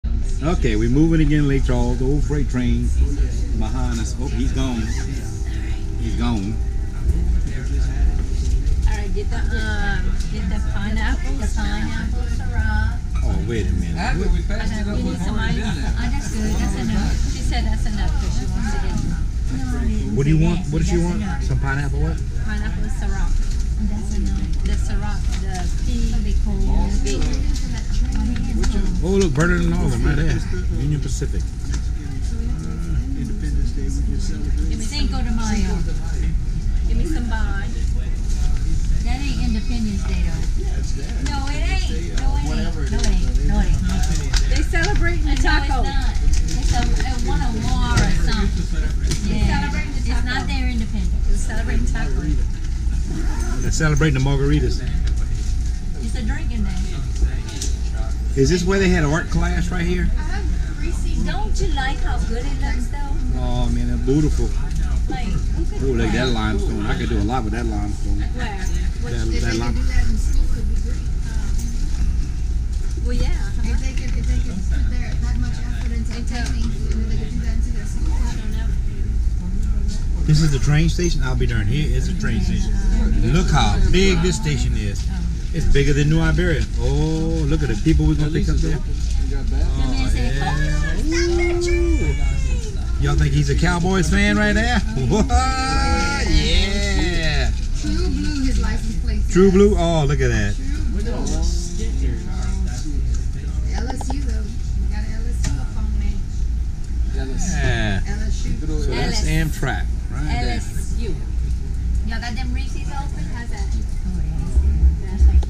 Amtrak trip from New Iberia, LA to San Antonio,  TX